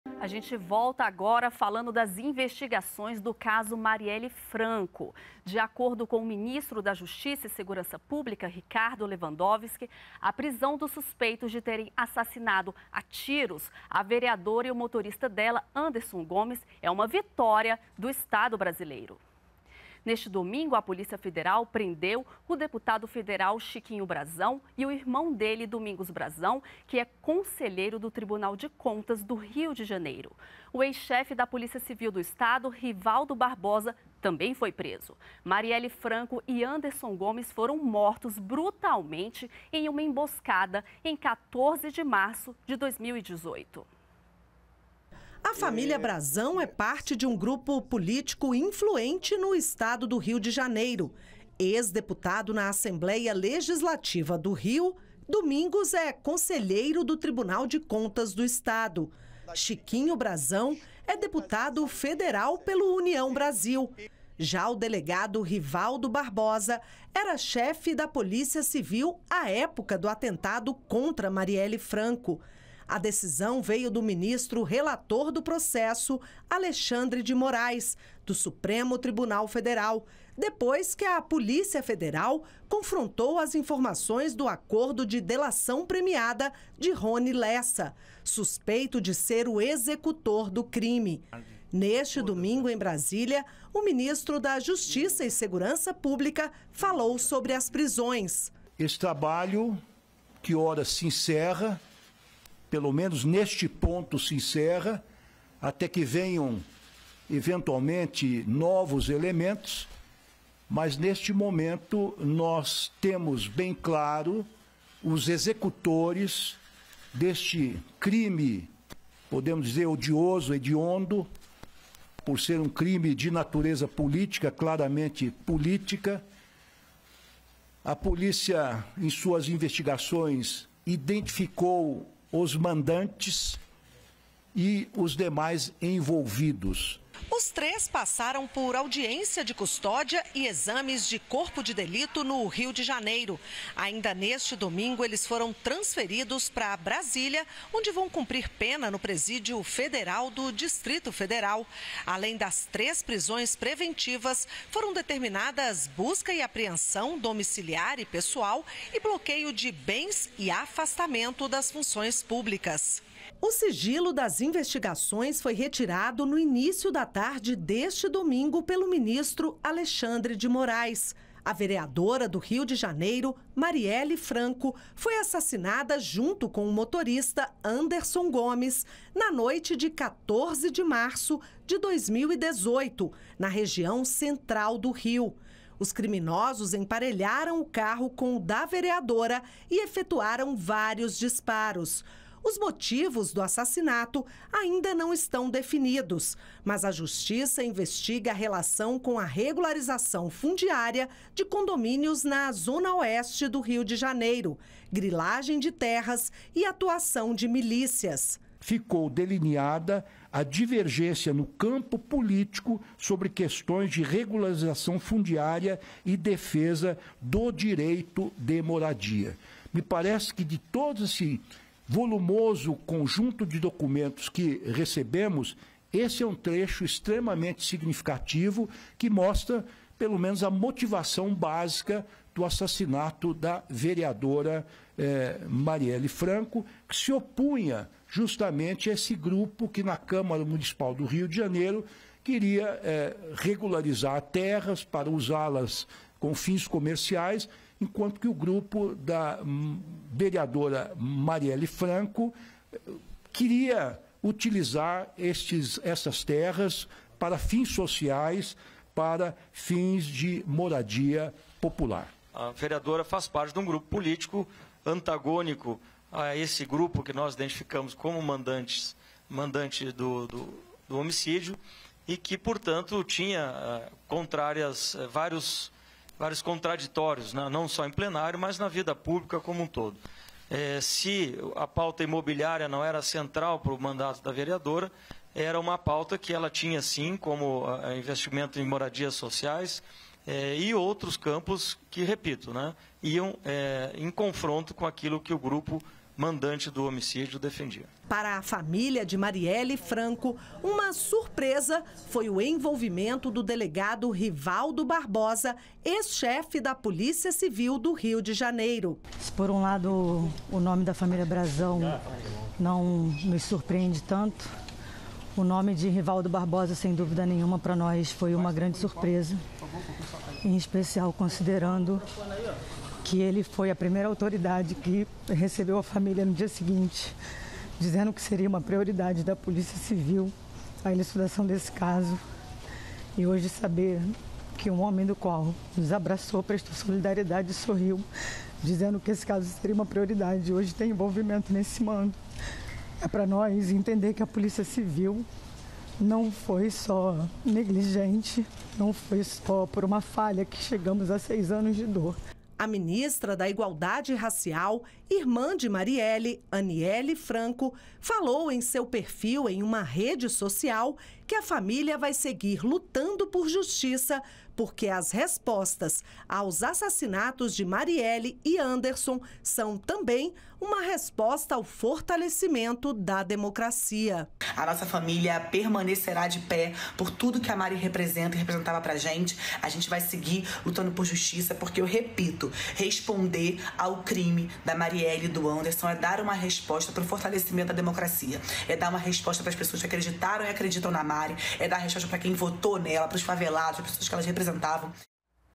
LEWANDOWSKI: PRISÃO DE ENVOLVIDOS NO ASSASSINATO DE MARIELLE FRANCO É VITÓRIA DO ESTADO BRASILEIRO
O ministro da Justiça e Segurança Pública, Ricardo Lewandowski, disse que prisão de envolvidos no assassinato de Marielle Franco é vitória do Estado brasileiro. No domingo (24), foram presos os irmãos Chiquinho e Domingos Brazão, além do ex-chefe da Polícia Civil do RJ, Rivaldo Barbosa.